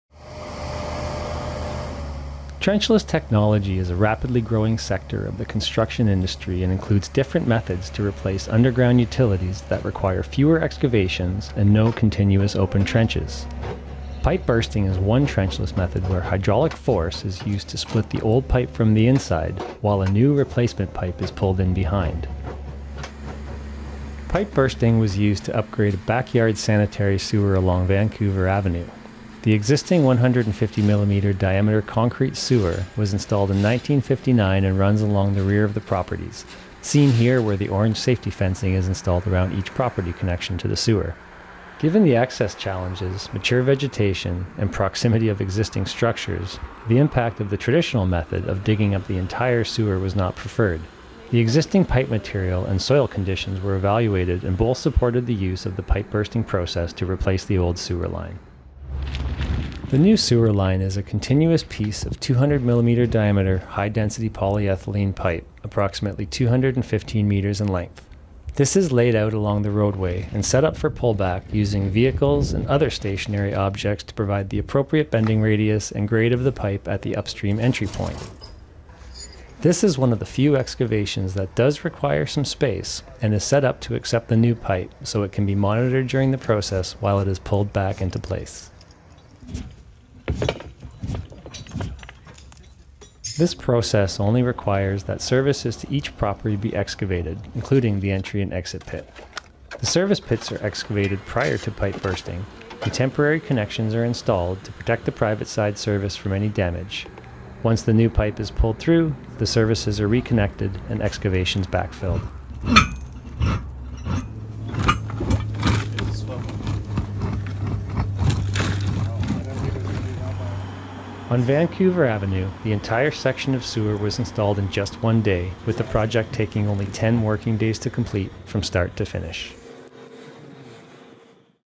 Pipe Bursting on Vancouver Ave (CITY of NANAIMO)
When old concrete piping in Nanaimo needs replacing, digging it up is no longer the only option. Pipe bursting, which breaks through old pipe by pulling a new pipe through it, is now a better and lower-cost option than trenching through a neighbourhood's backyards.